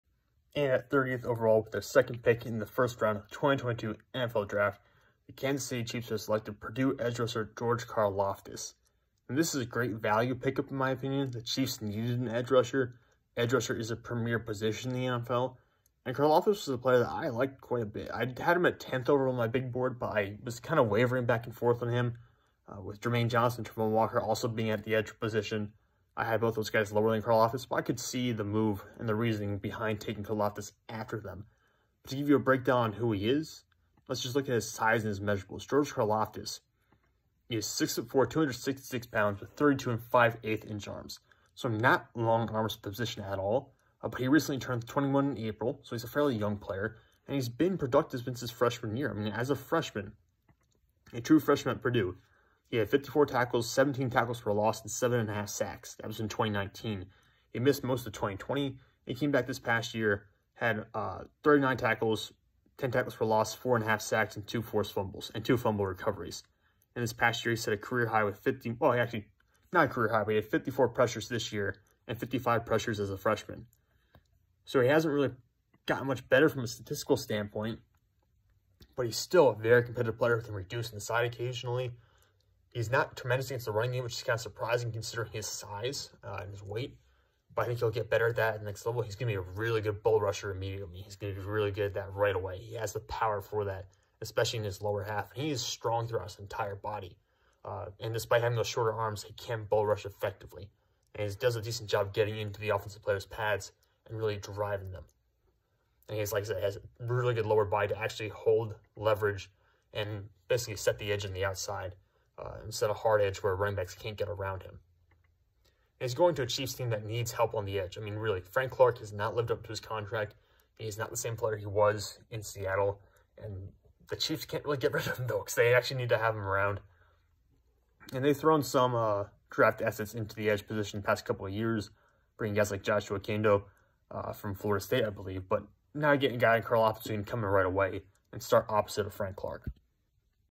Reaction to Kansas City Chiefs drafting George Karlaftis
George Karlaftis, EDGE Purdue

Size: 6'4", 266 lbs. with 32 5/8" arms

Pros: Turned 21 in early April, consistent producer since his freshman season, combined 109 pressures in 2019 and 2021, built like a brick house, can reduce inside and work from several alignments, bull rusher, shocks linemen with his initial punch, doesn’t wear down as the game goes on, active hands are always working to create separation or establish a point for him to display his power, has the lower body strength to set the edge

Cons: Arm length and wingspan, 20 missed tackles in 26 games, surprising lack of production in the running game, didn’t record a pressure against Ohio State (2021), cornering and overall agility are average at best

Production: 39 tackles, ten tackles for loss, 4.5 sacks, and two forced fumbles in 2021

Production: 54 tackles, 17 tackles for loss, 7.5 sacks, and one forced fumble in 2019